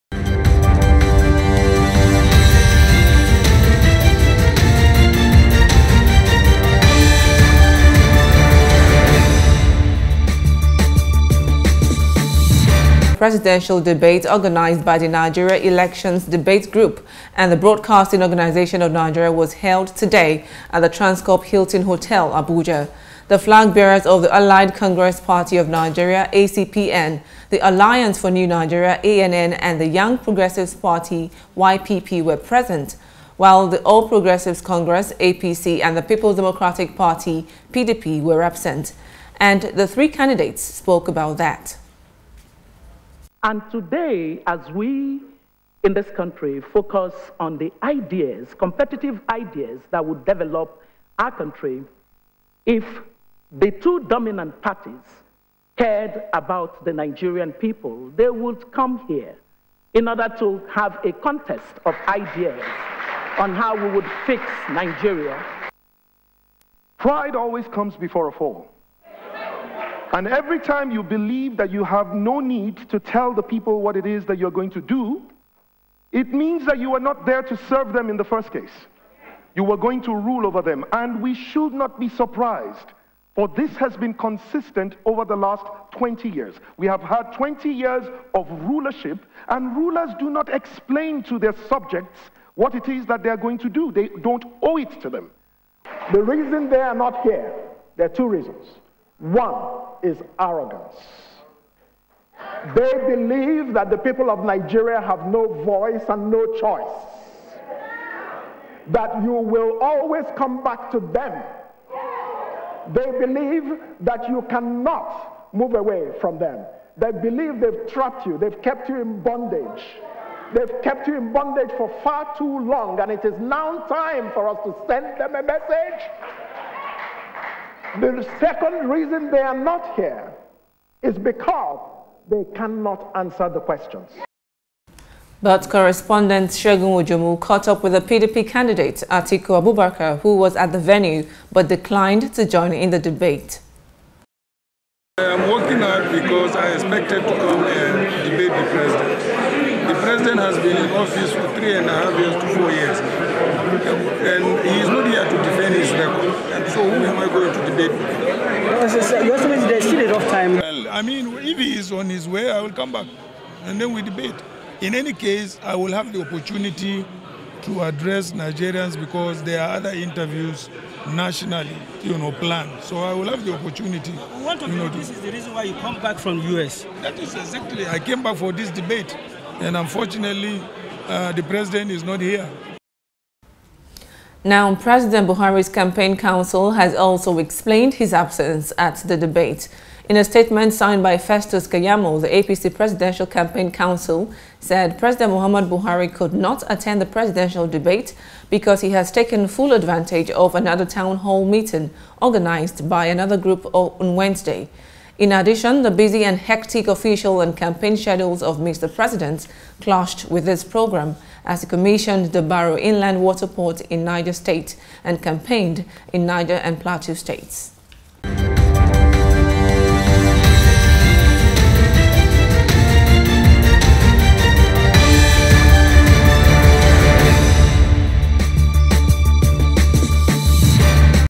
2019 Presidential Debate
Five Political Parties where involved in the debate but only three parties participated in the Presidential debate - the ACPN, ANN and YPP. Ruling Party, APC's Presidential Candidate President Buhari was absent as well as the candidate of the People's Democratic Party, PDP.